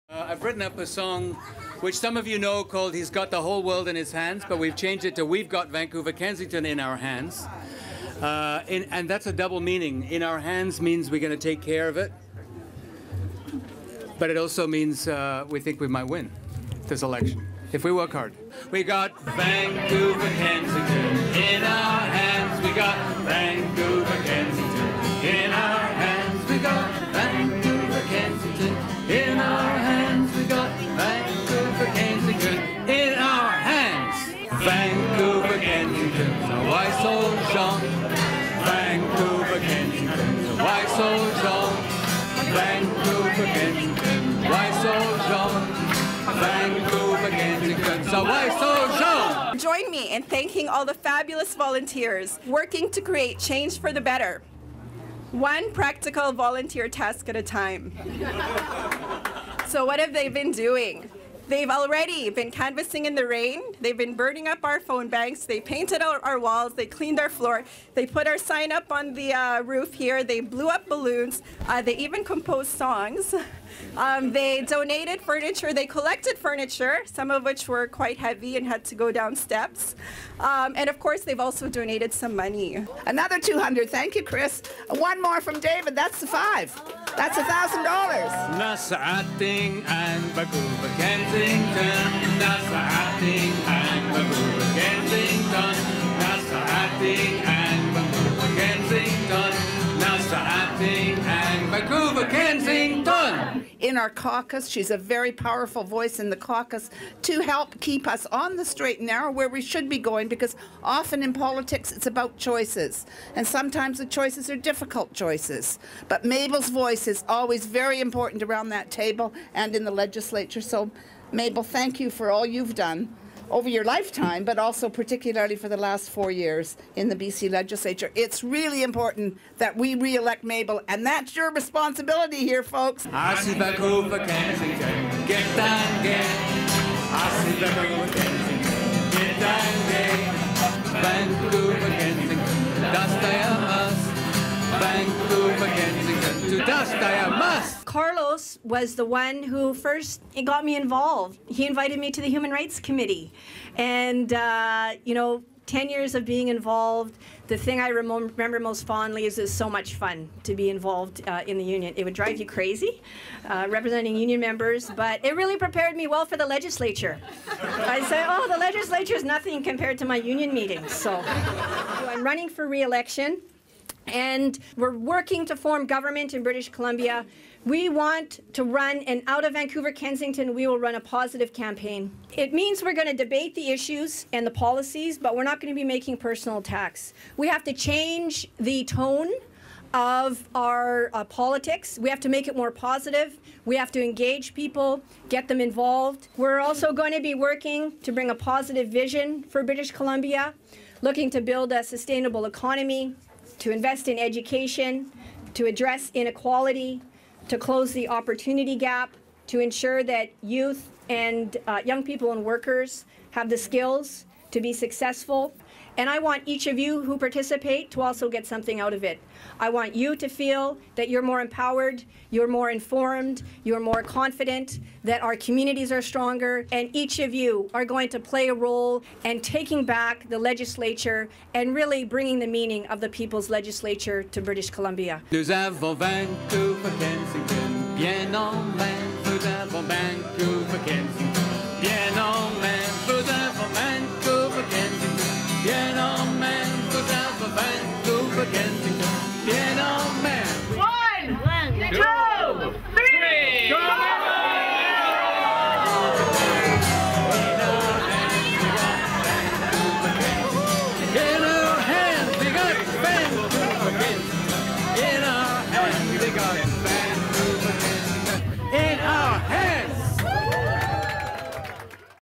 Vancouver-Kensington Campaign Office Launch
Mable Elmore, the NDP candidate for Vancouver-Kensington, officially opened her campaign office to a packed house of supporters on March 30th. Lots of food, laughs, community singing and ribbon cutting! What a wonderful and vibrant start to the campaign!